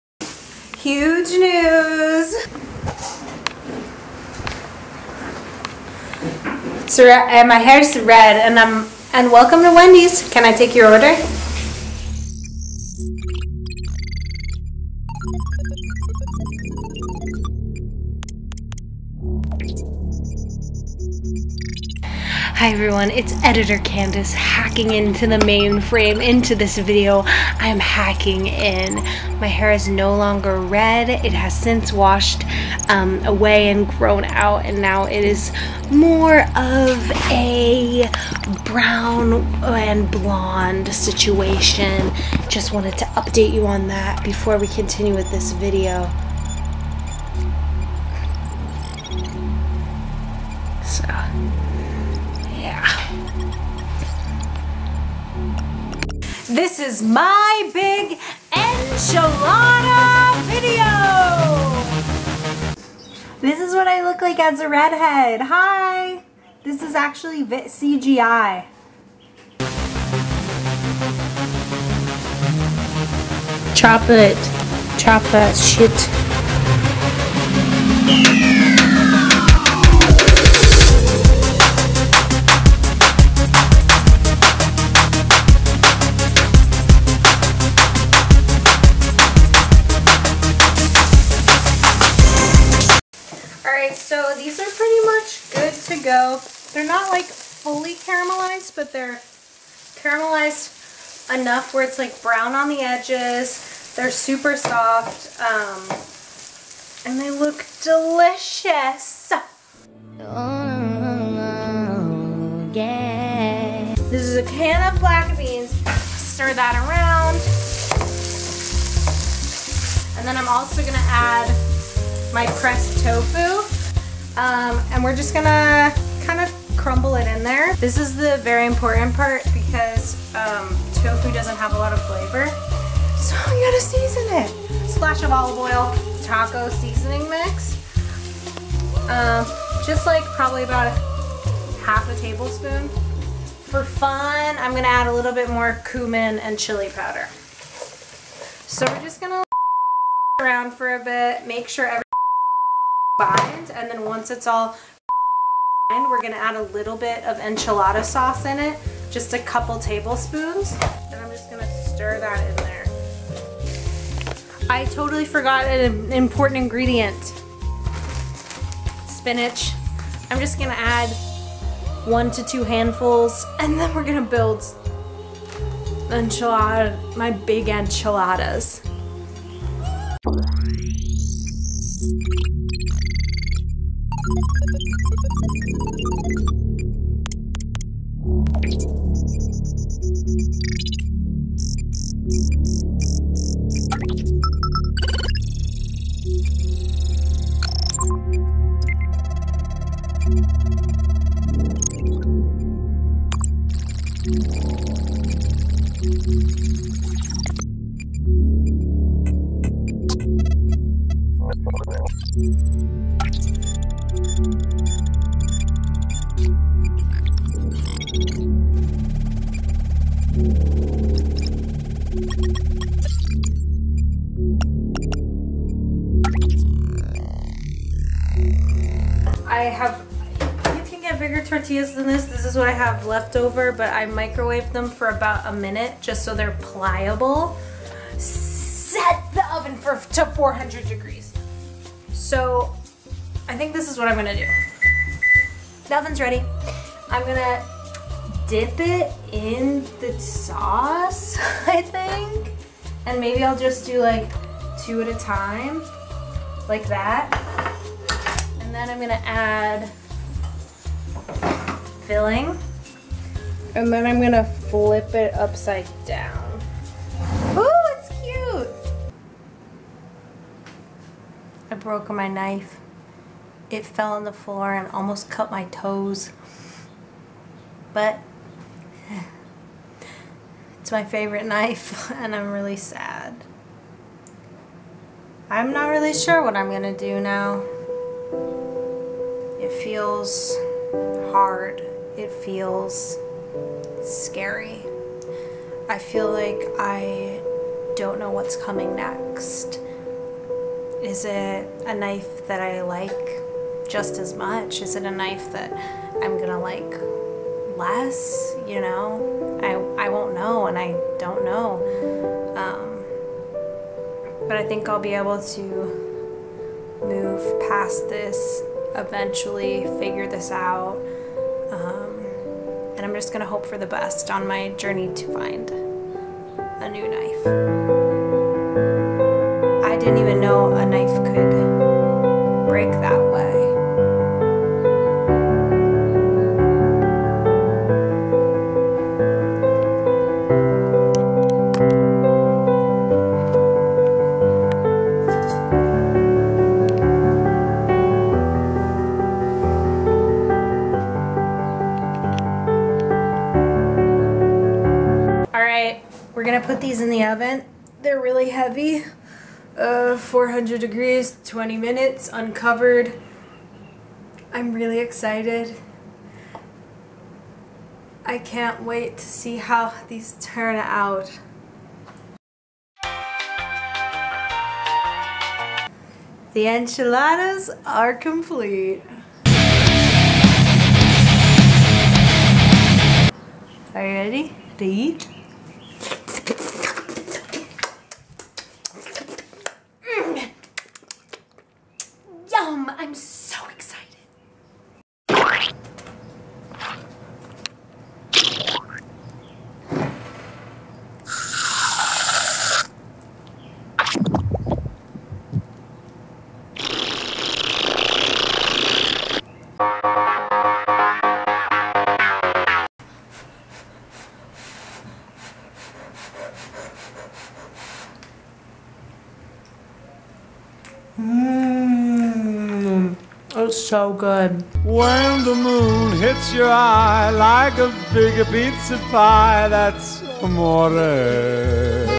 MY BIG ENCHILADA VIDEO
Recipe Below!

Ingredients:
1 Block of Firm Pressed Tofu
1 Can of Black Beans Drained & Rinsed
1 Onion
2-3 Garlic Cloves
1 Red Bell Pepper
Spinach
Taco Seasoning
2 Bottles of Enchilada Sauce
Shredded Cheese
Salt
Pepper

Recipe:
- Slice the bell pepper, onion, and garlic into thin strips so they cook quickly.
- Heat olive oil over medium high heat then add the vegetables
- Add salt and pepper and a sprinkling of taco seasoning
- Cook the veggie for about 10-15 minutes until very soft and a little golden
- Then add your black beans and pressed tofu. Stir the mixture around breaking up the tofu into small pieces.
- Add a splash more olive oil, a half tablespoon of taco seasoning and any other seasonings you like! You can try oregano, cumin, or chili powder!
- Once stirred, add about 3 tablespoons of enchilada sauce and simmer for 5 minutes.
- While the enchilada filling is simmering, microwave your tortillas for about 1-2 minutes so they are pliable
- Dip both sides of the tortilla in a thin layer of sauce then add the filling and shredded cheese. Fold the tortilla from left to right and flip upside down so they stay closed. Do this until you use all your filling.
- Add extra sauce and cheese on top of your prepared enchiladas.
- Put them in the oven uncovered for 20 minutes!